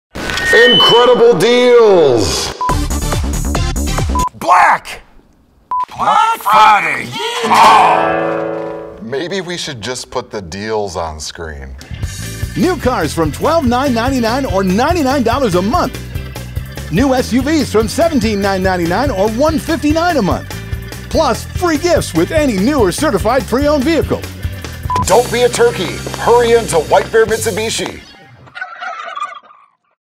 White Bear Mitsubishi - November Commercial
White Bear Mitsubishi in White Bear Lake, MN, is offering customers an early Christmas gift in 2020. All month long, you can save up to $8,000 on the purchase of your dream car. They are also offering leases starting as low as an incredible $49 a month! Stop in today to find your new ride and save some Christmas Cash.